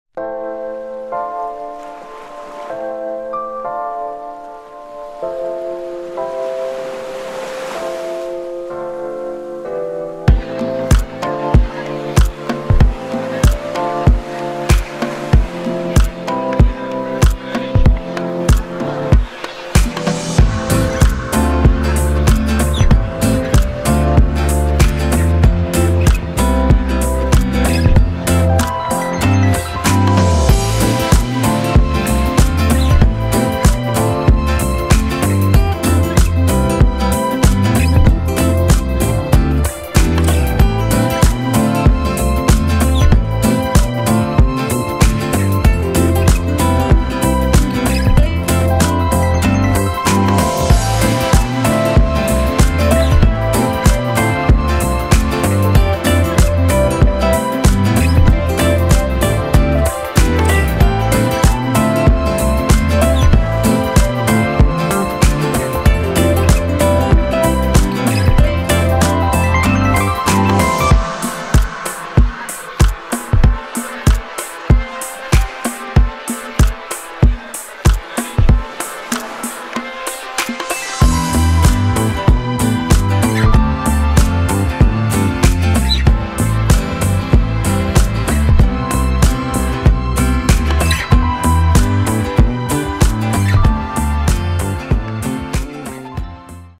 Carvoeiro 4 bedroom sea view villa with pool for sale near the centre and beaches in West Algarve
Discover this stunning spacious villa located in Carvoeiro, Portugal! This beautiful property is just a short walk from the beach and a golf course, offering breathtaking sea views. Perfect for families or anyone seeking a relaxing getaway, this villa combines luxury with convenience. Join us as we explore the features, layout, and stunning surroundings of this dream home.